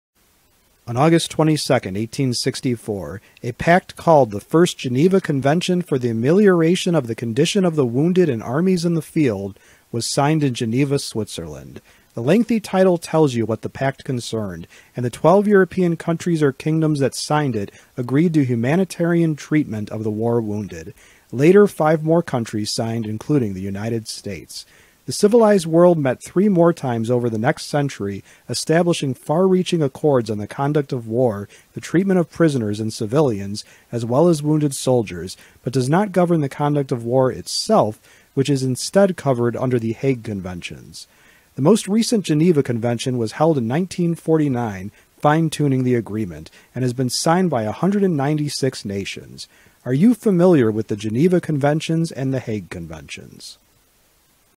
History Short: What is the Geneva Convention?
On August 22, 1864, the first of 4 treaties governing the conduct of war was signed in Geneva, Switzerland, a pact called “The First Geneva Convention for the Amelioration of the Condition of the Wounded in Armies in the Field.”

The text narrated in this video is from the original article